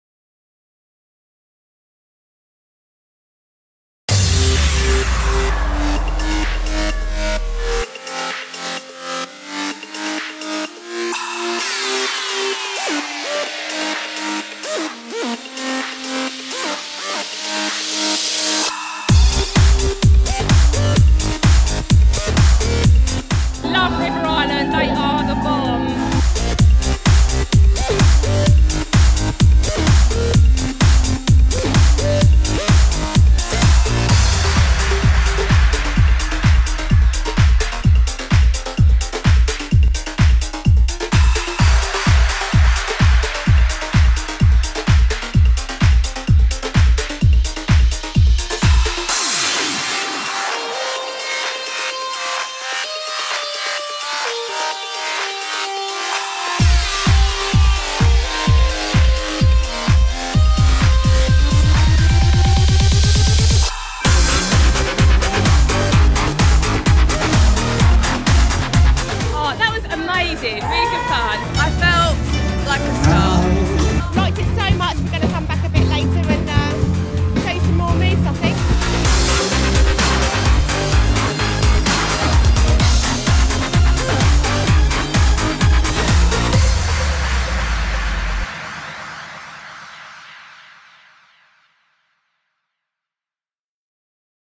River Island singINstyle at Isle Of Wight Festival 2013
We were at Isle of Wight Festival this year to give festival goers the chance to perform like a headline act at our very own singINstyle tent.

See how everyone transformed themselves in our stage-ready outfits and flexed their vocal chords by hitting play!